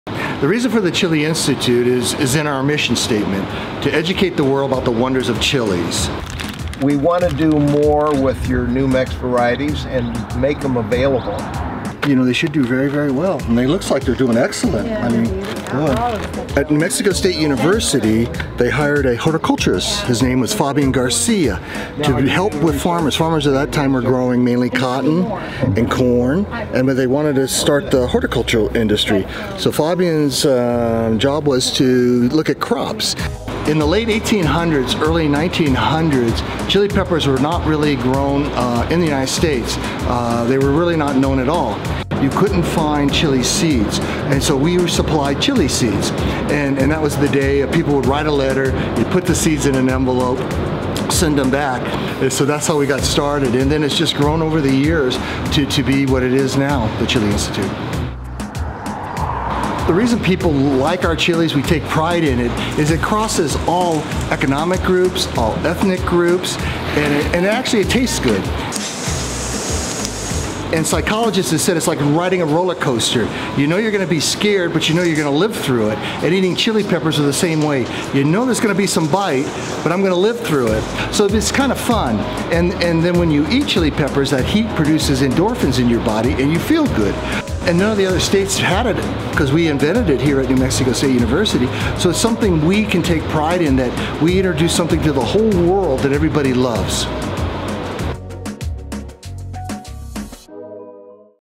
The Chile Pepper Institute @ New Mexico State University
Nowhere is chile (yes, spelled with an "e") more tied to culture and cuisine than in New Mexico.
And nowhere can take more credit for it, or has more knowledge about it, than New Mexico State University, home to The Chile Pepper Institute.
A visit will garner you a walk through the gardens and a chance to learn about how this iconic New Mexican crop came to be.